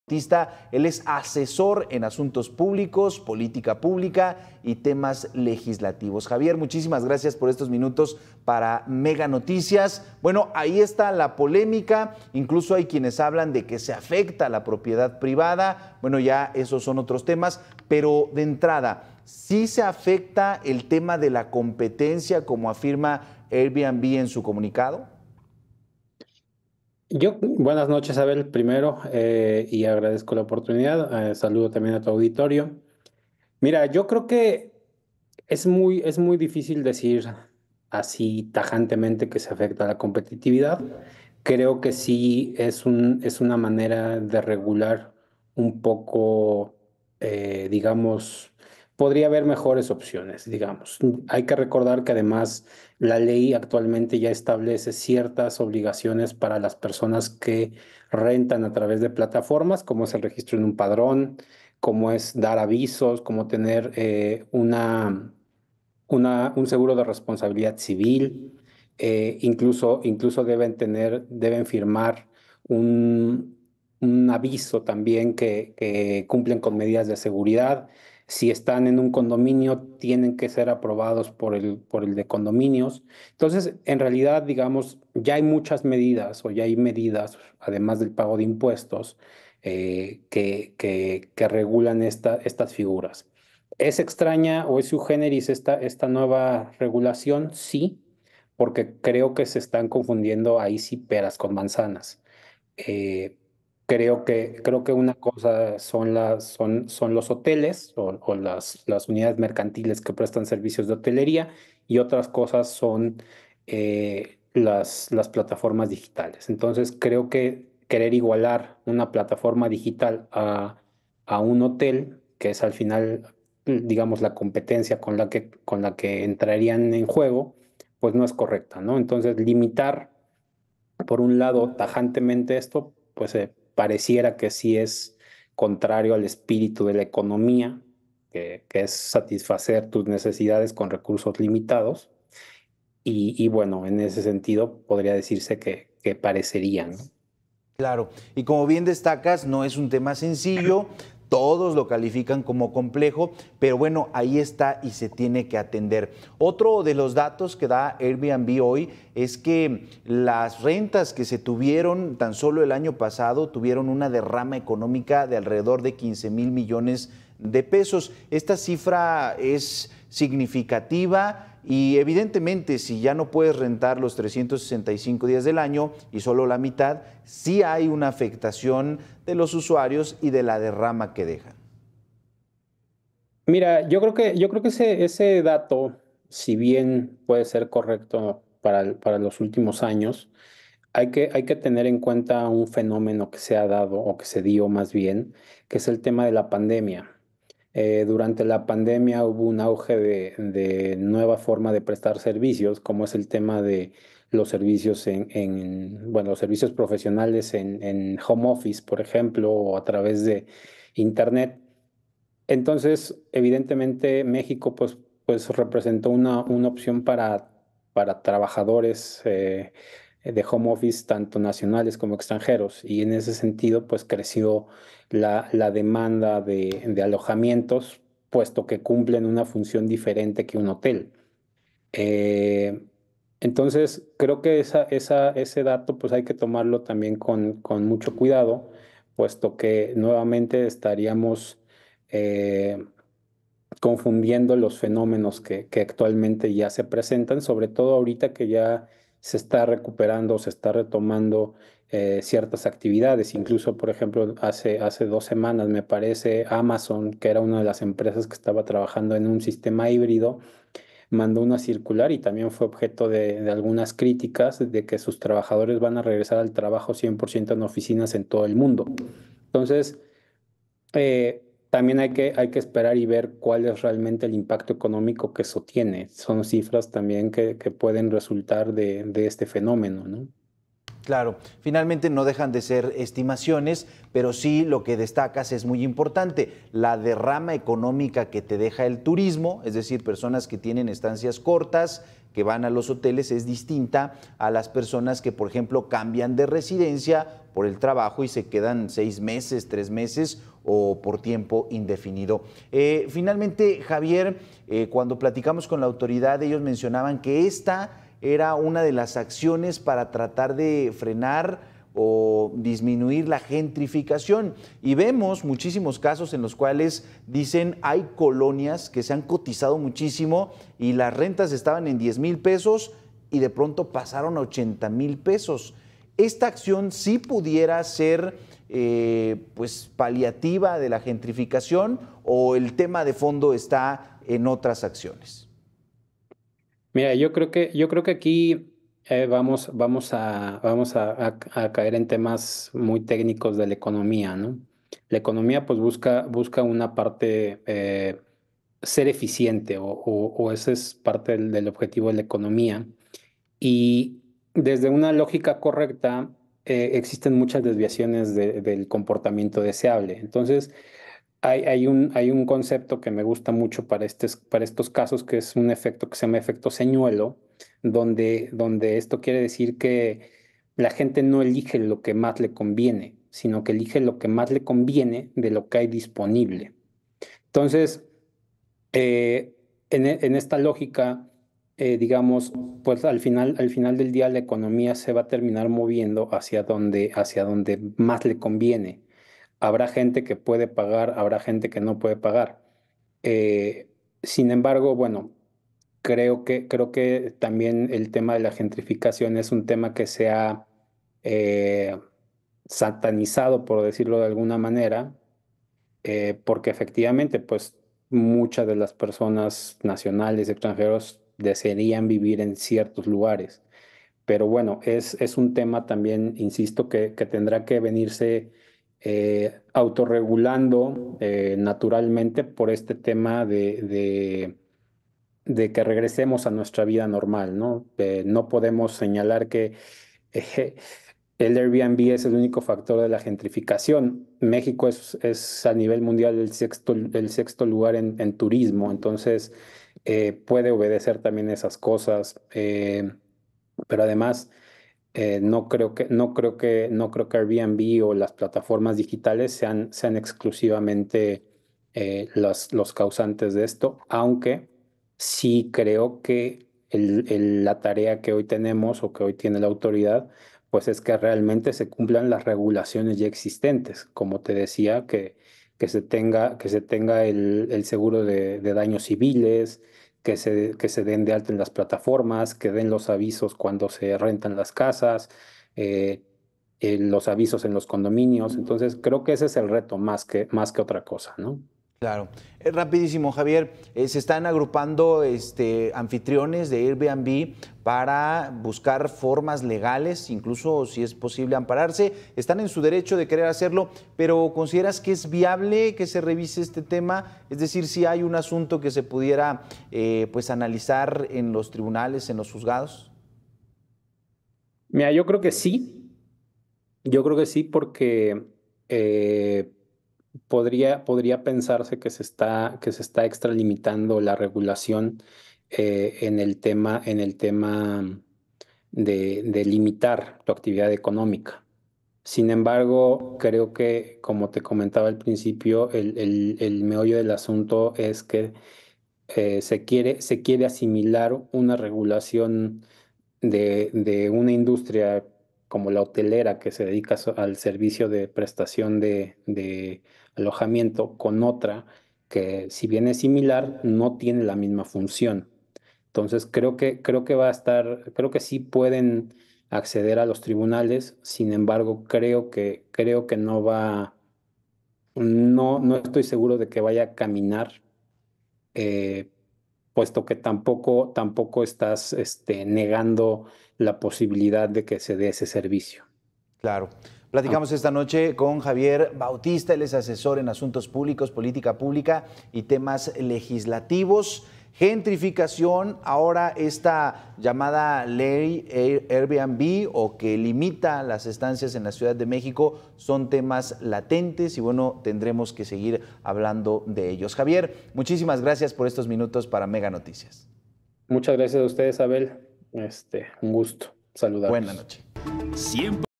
Se sataniza la gentrificación; plataformas como Airbnb no son las únicas causantes del fenómeno
Regular las aplicaciones como Airbnb no se puede decir de manera tajante que afecta la competitividad. Las plataformas digitales no se puede comparar con los hoteles y limitarlas parece que afecta la competitividad. En la pandemia hubo auge de prestar servicios como el home office y México representó una opción en este sentido, por lo que se debe esperar el impacto económico que tienen, así lo comentó Javier Bautista en entrevista con Abel Martínez.